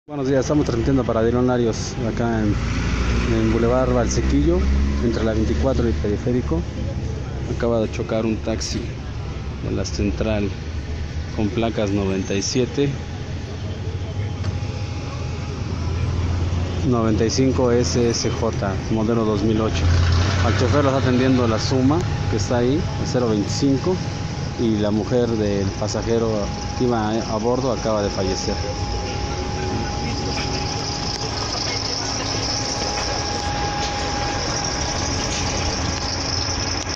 Fallece fémina en Puebla tras choque entre taxi y ruta del transporte público
Mujer de 39 años murió la mañana de hoy a las 10:20 AM aproximadamente el conductor hiba en edo de ebriedad y el taxi no tenía póliza de seguro